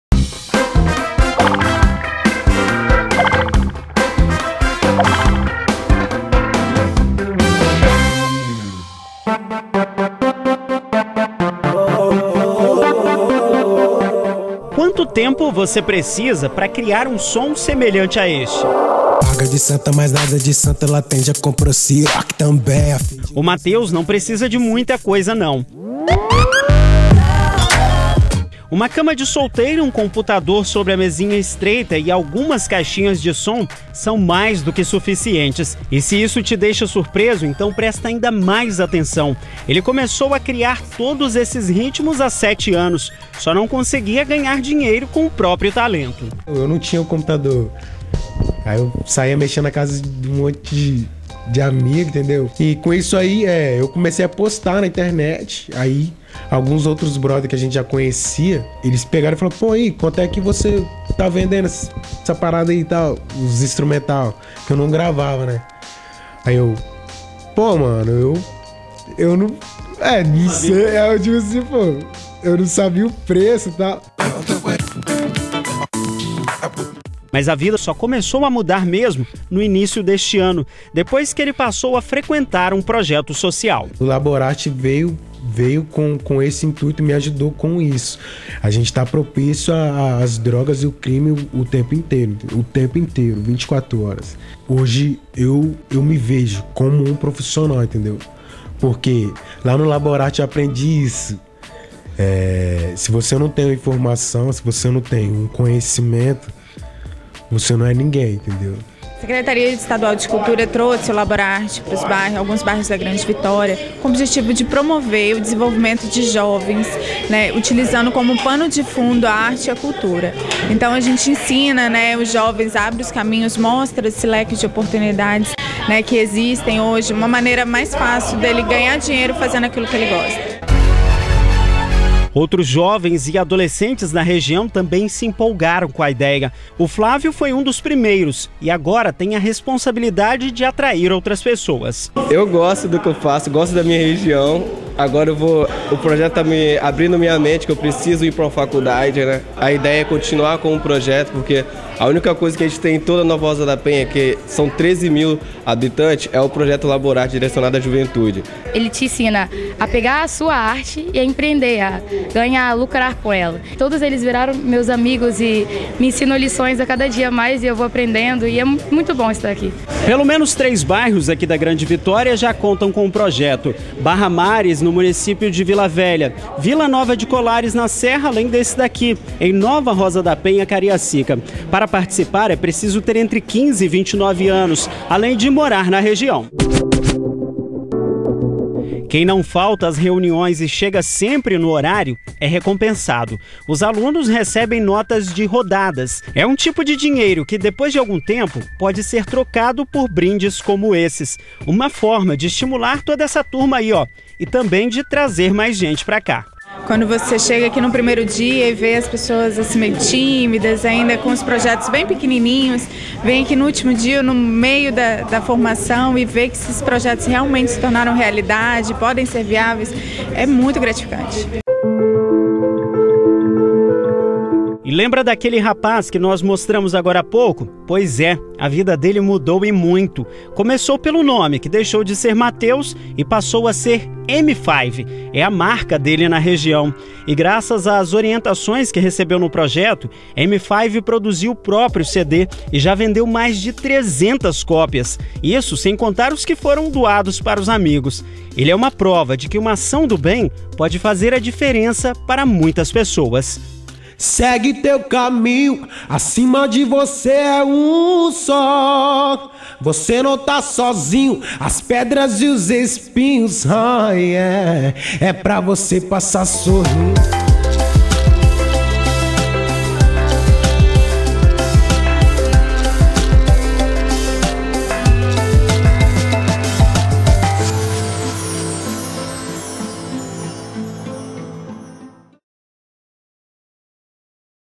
Gentileza, eu curto
Um projeto social presente em bairros da Grande Vitória está mudando a vida de muitos jovens. Ficou curioso? Então sente a batida do mfive, de Boa Esperança, Cariacica.

Nota: Tem boas notícias para os jovens da Grande Vitória. As inscrições para o laborarte estão abertas. São duzentas vagas. Para mais informações, é só entrar no endereço do facebook